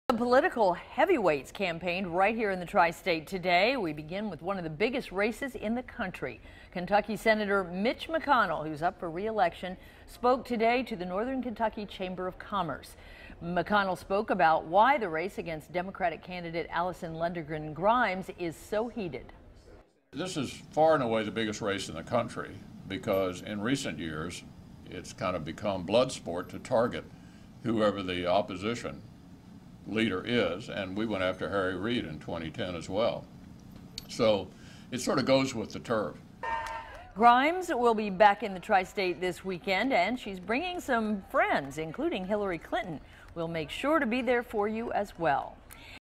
Senate rivals woo Northern Ky.
Mitch McConnell calls attacking opposition leader "bloodsport."